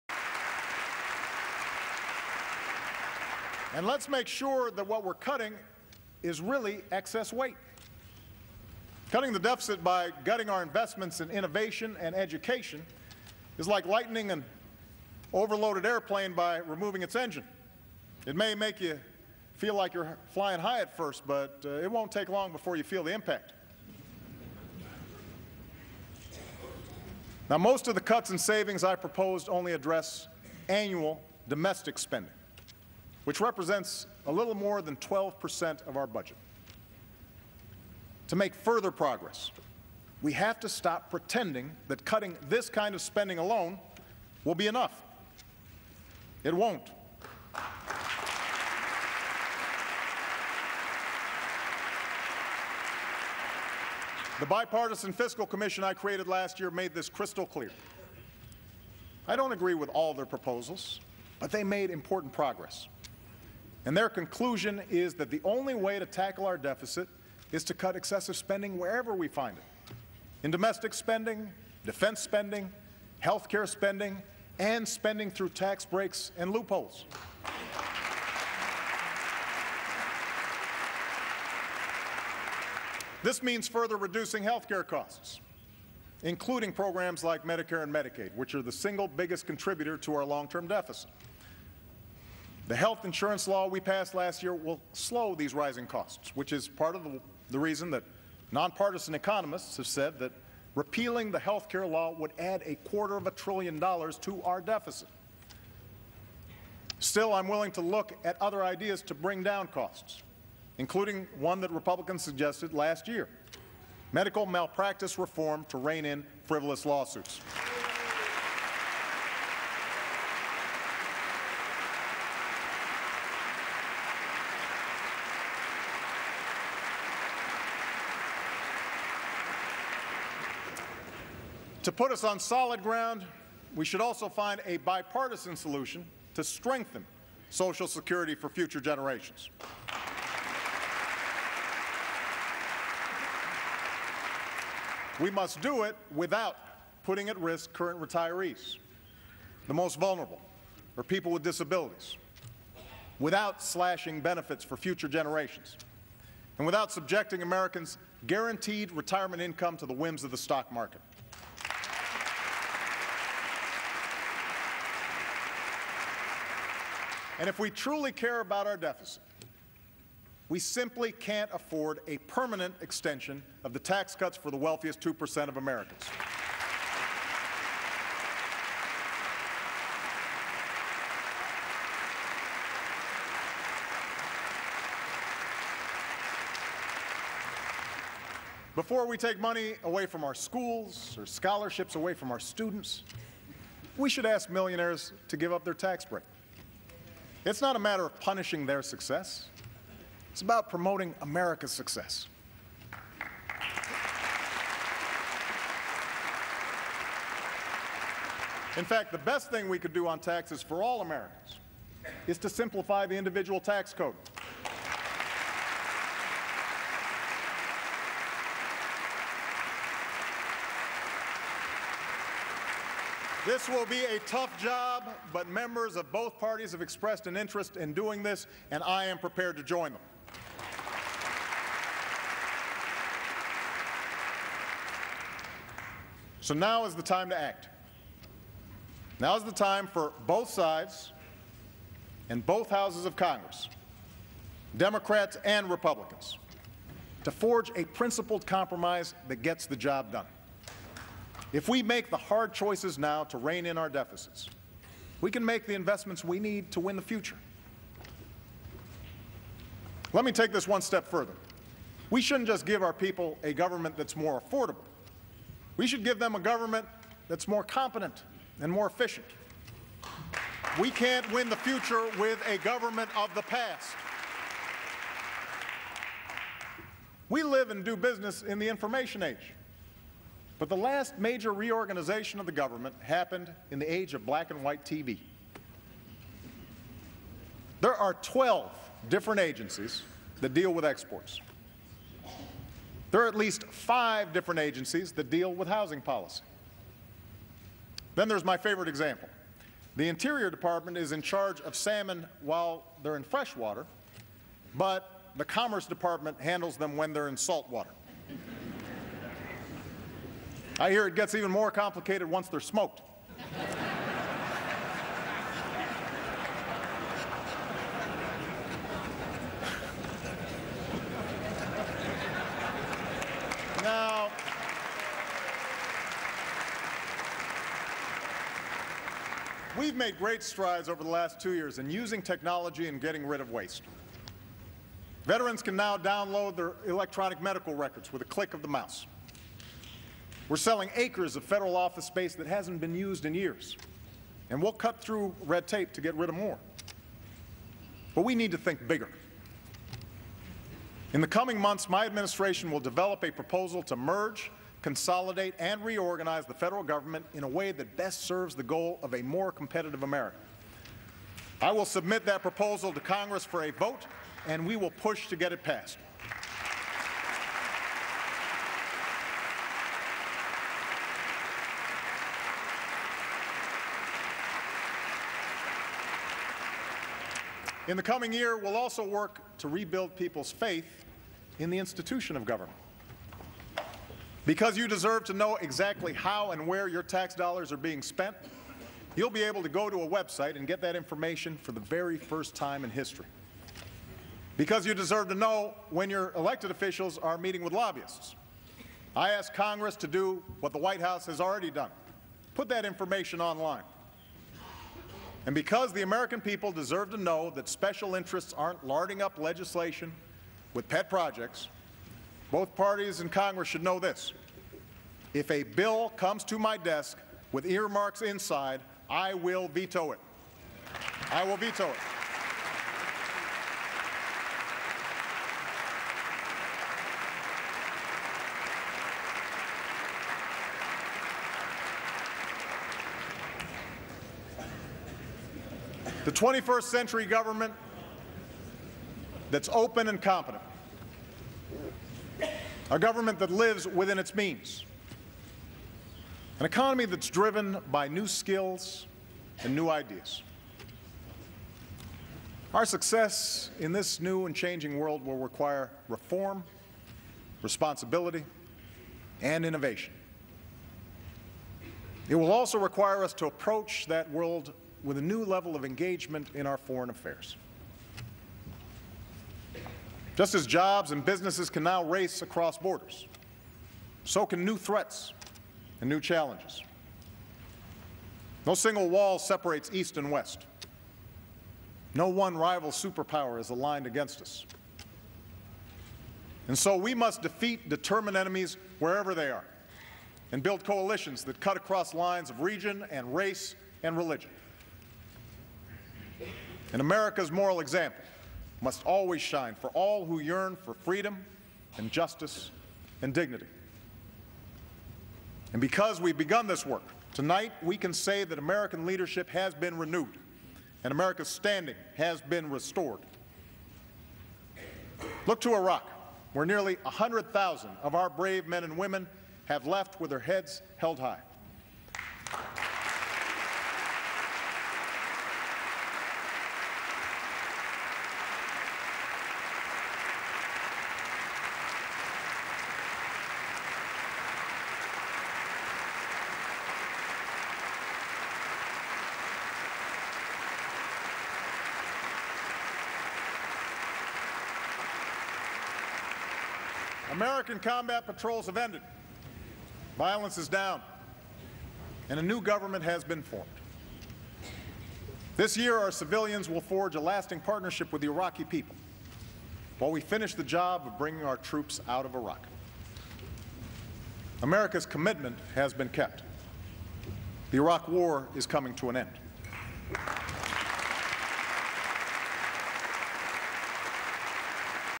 2011 State Of The Union Part 5
President Barack Obama gives the 2010 State of the Union Address to the nation on January 25, 2011.  This is part 5 of 7